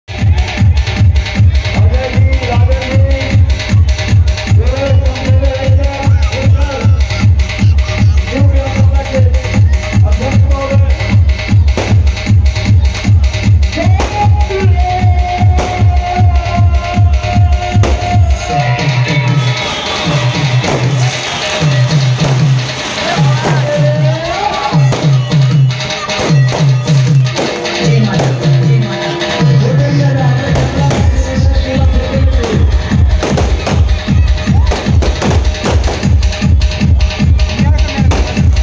kings club ramnavami 2023light house darbhanaga bihar
hi-tech dj shivam darbhanaga bihar ramnavami dj setup in Bihar darbhanaga

kings club ramnavami celebration in Darbhanga bihar